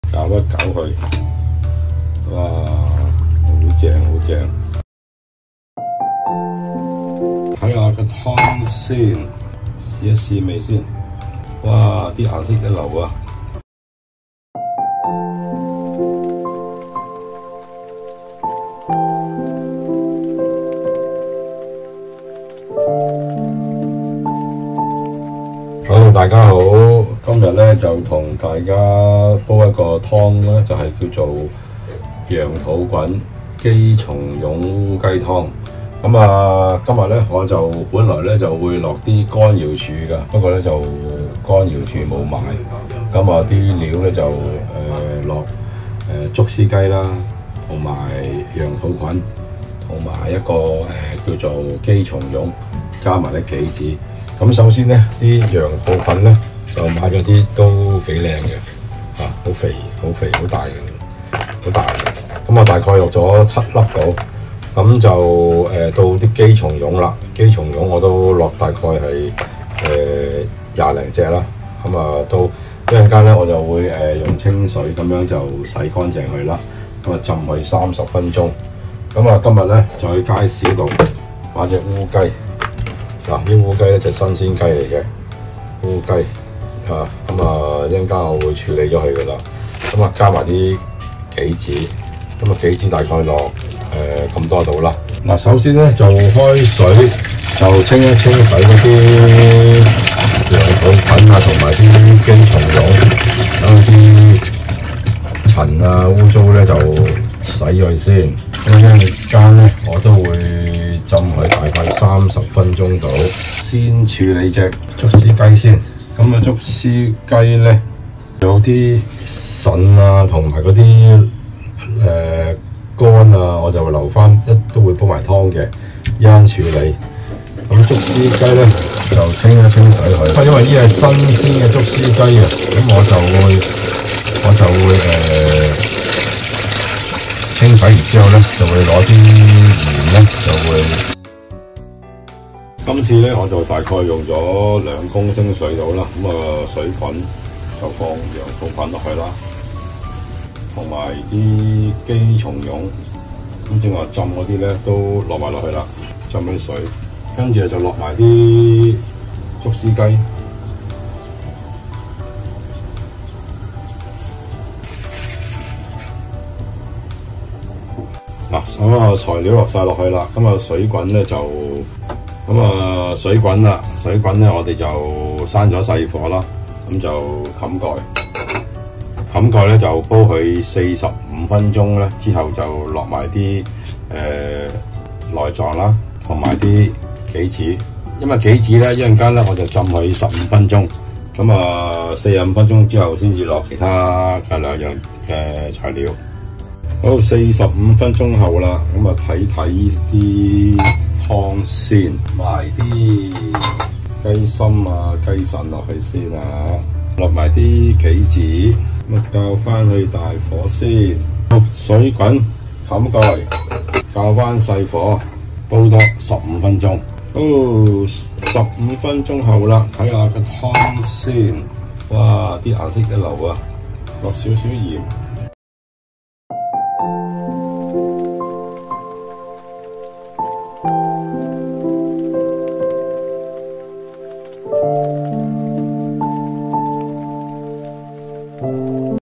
羊肚菌姫松茸烏雞湯｜化痰理氣｜補腦提神｜補腎｜有益腸胃｜抗衰老、抗突變、抗腫瘤
料理：
竹絲雞
羊肚菌
姬松茸
杞子

處理：
羊肚菌姬松茸沖洗乾淨浸清水30分鐘、
竹絲雞清洗內臟、用粗鹽冲刷新雞身、然後用清水沖洗乾淨、斬件

制作、
煲滾2公升水、放入材料、先放入乾瑤柱連水、羊肚菌姬松茸連水放入、放入雞、內臟稍後放入、
煲滾、攪勻、冚蓋煲轉小火煲45分鐘
杞子浸15分鐘、
45分鐘後放入內臟及杞子、然後用小火煲多15分鐘、完成。

羊肚菌～
和胃消食有效增強免疫力
羊肚菌性平味甘，功效多樣，具有化痰理氣、補腦提神、補腎及增強免疫力等功效，平日多食用羊肚菌燉湯能夠強身健體，有益腸胃，對於紓緩食積氣滯、消除焦慮等有幫助。

姬松茸～
通過美國食品及藥物管理局認可，證實是一種能提高免疫力的茸類，功效比靈芝更高，被譽為「菌中之王」。日本專家在動物實驗中證明，姬松茸對抗病及提升免疫力的實際功能，遠勝其他菌類。

竹絲雞～
全身毛白色，因皮、肉、骨嘴均為黑色，故而得名烏骨雞，是中國著名的藥用珍禽之一。 竹絲雞性平、味甘; 具有滋陰清熱、補肝益腎、健脾止瀉、調經等作用。 適宜作為煨湯、炖食之用。 全年均有供應。

杞子～
主要功效為補肝腎、明目。 枸杞營養豐富，富含甜菜鹼、多糖、粗脂肪、粗蛋白、硫胺素、核黃素、胡蘿蔔素、抗壞血酸、尼克酸，及鈣、磷、鐵、鋅等元素。 具有增強網狀內皮系統吞噬能力，有增強細胞與體液免疫的作用；對造血功能有促進作用；還能抗衰老、抗突變、抗腫瘤、保肝及降血糖等好處。